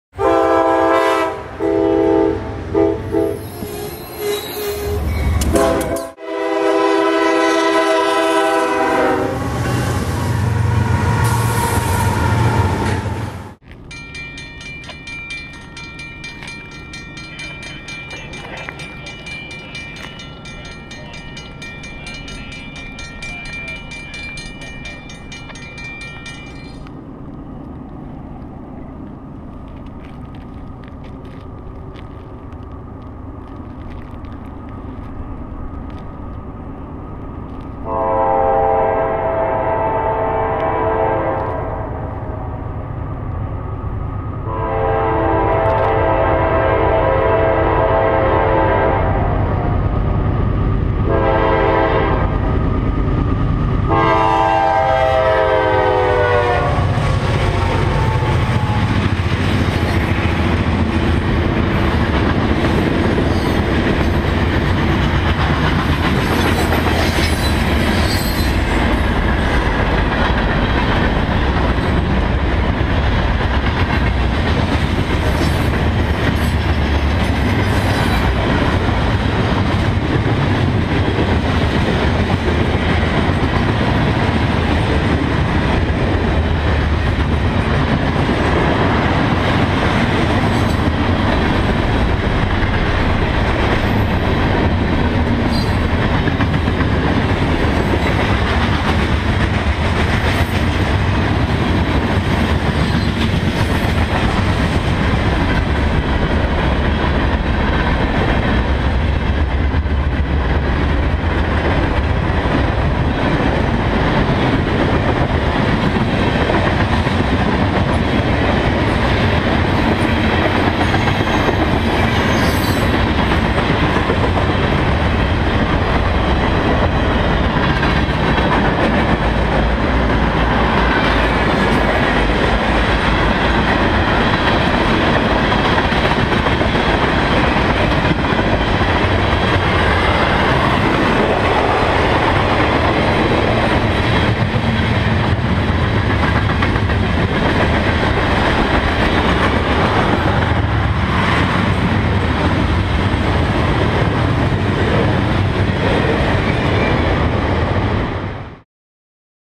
CSX in Kirkville, NY (CSX Q626)
Out with my great friend Tyler in Syracuse, NY.  Train 1 of our day is CSX Q626.